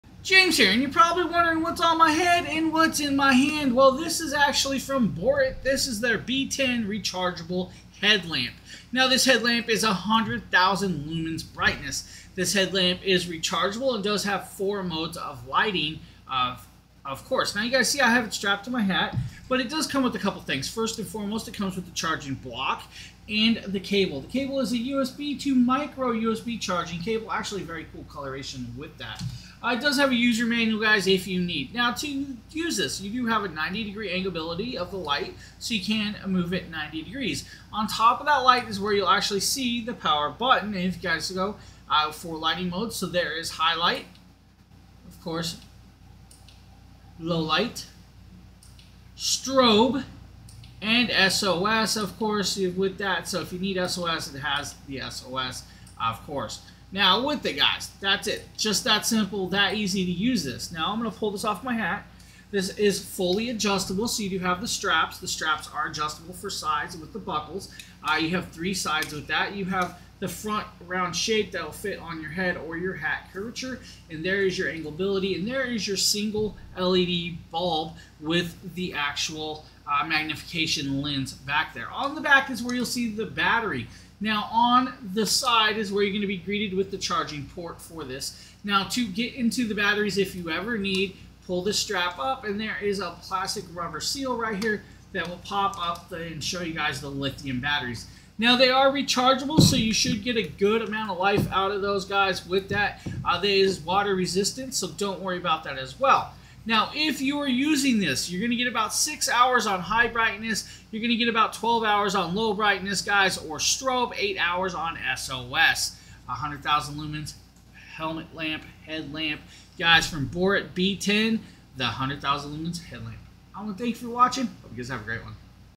How Bright is BORUIT B10 Rechargeable Headlamp
Check this out on Amazon

This BORUIT B10 is dfferent from other products they make, using a super bright high quality LED wick that can reach 100000 lumens, easily illuminating the road tents and mountains. With an effective life of up to 100,000 hours. There are 4 different modes to pick from including, high brightness(6H)-low light(12H)-strobe (8H)-SOS.